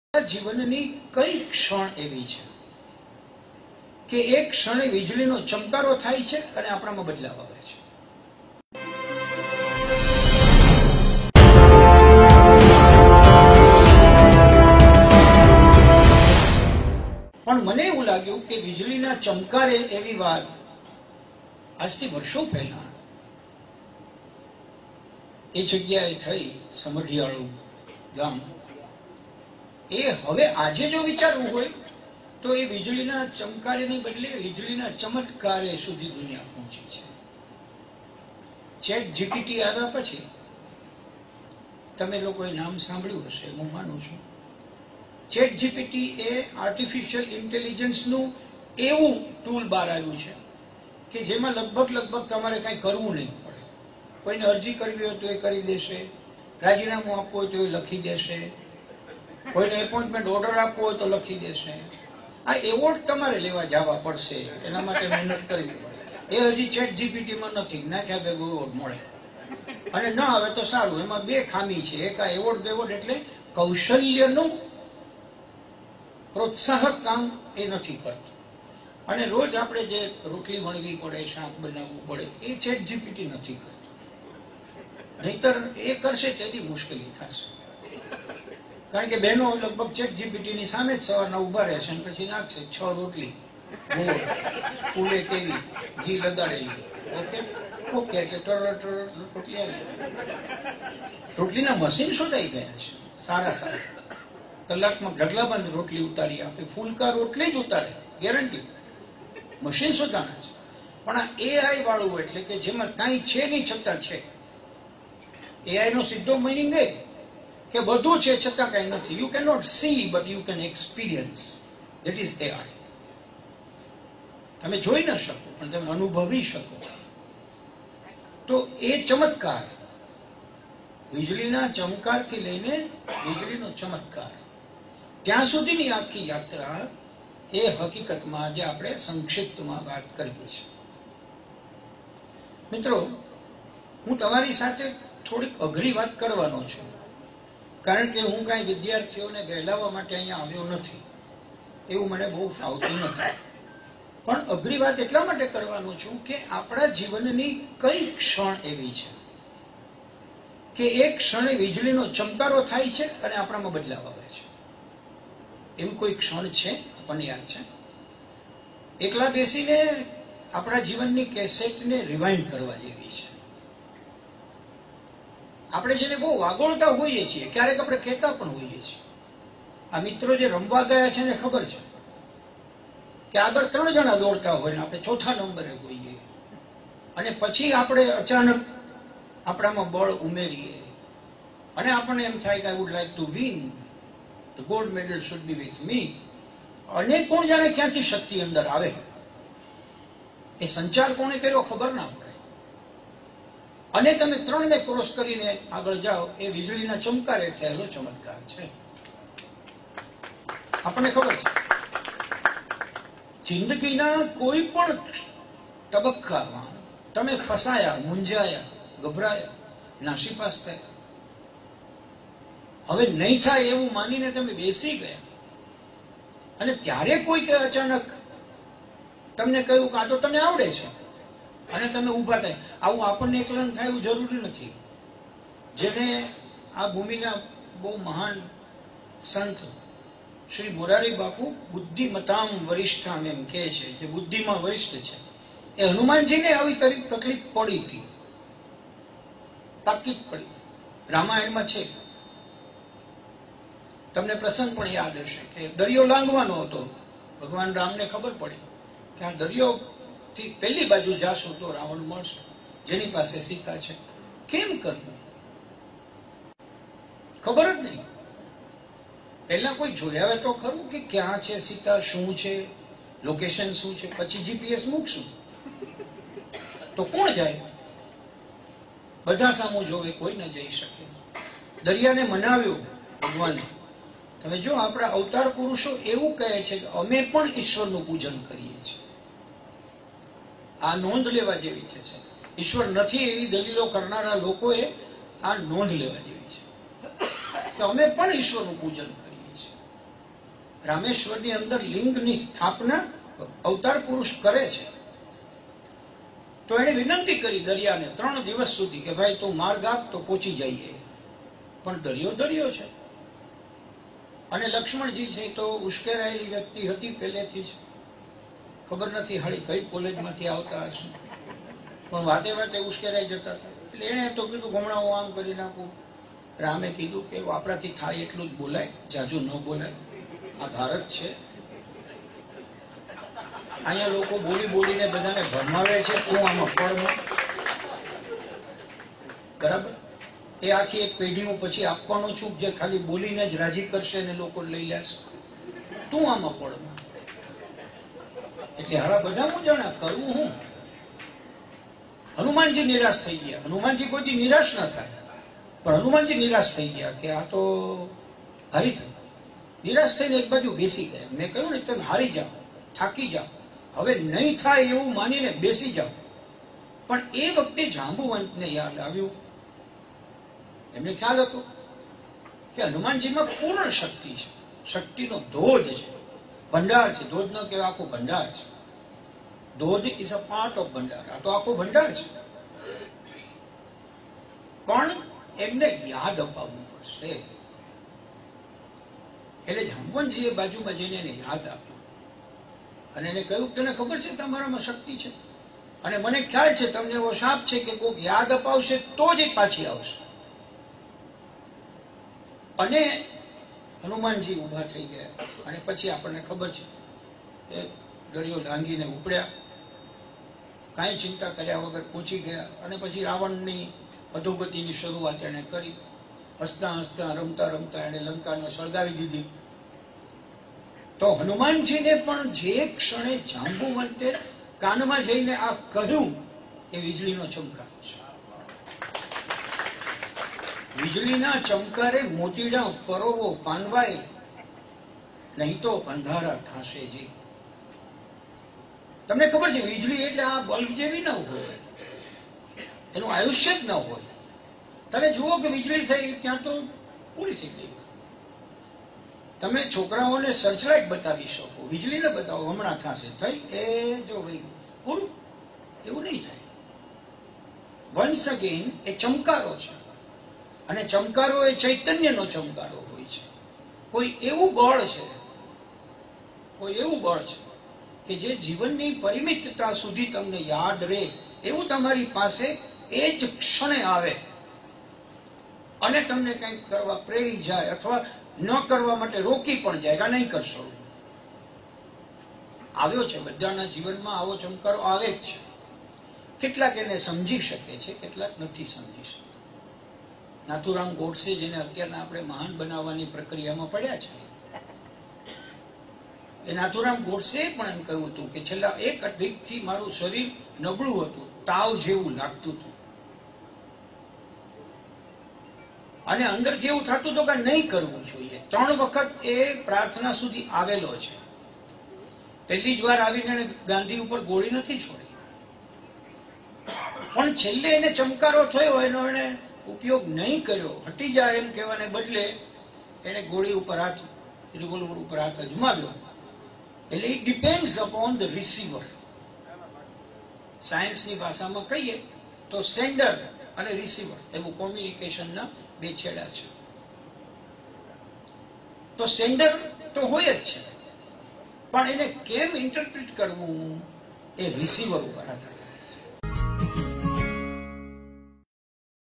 ||Bhadrayu@Science of Living|| khabarchhe:fb પર...(131)
શ્રેણીનો એકસો એકત્રીસમો મણકો::ઇશ્વર નથી એવી દલીલો કરનાર લોકોએ આ
નોંધ લેવા જેવી છે.

Bhadrayu Vachhrajani